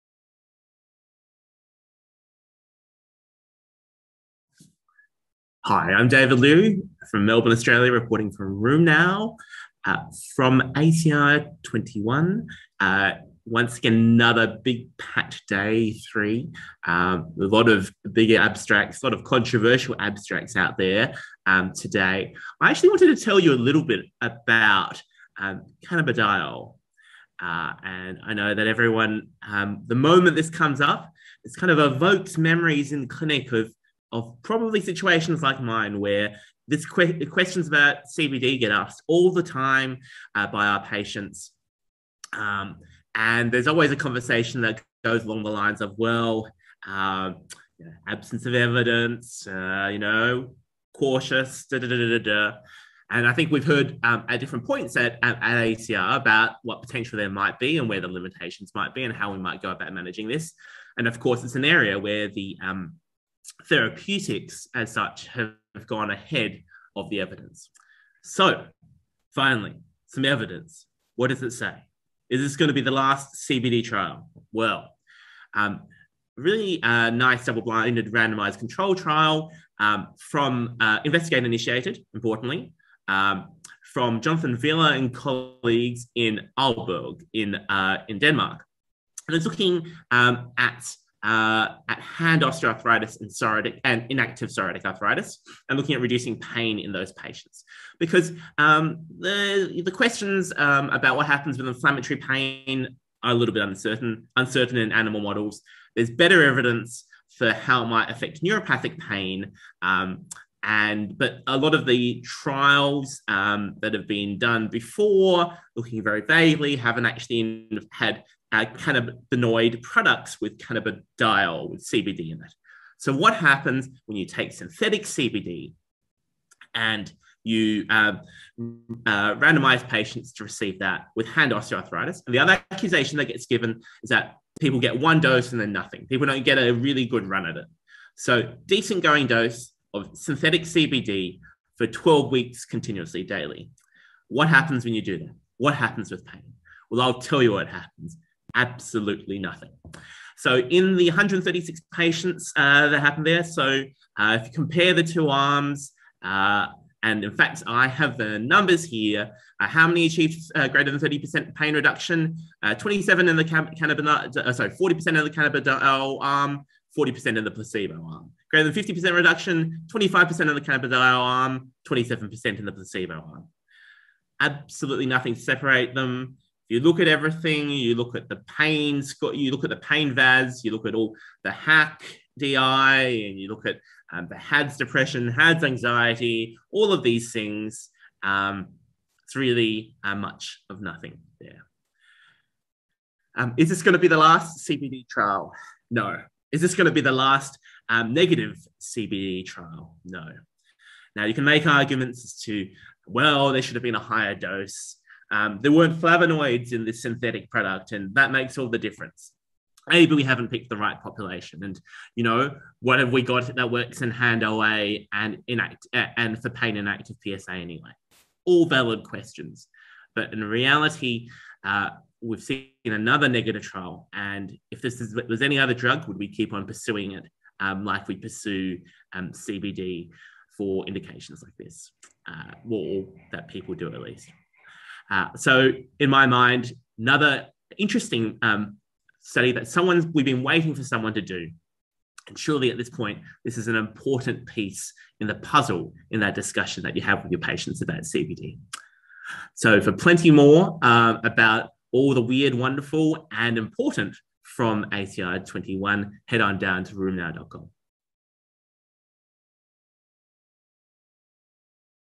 The Last CBD Trial: Dr. David Liew
Dr. David Liew discusses abstract #1456 presented at the ACR21 annual meeting.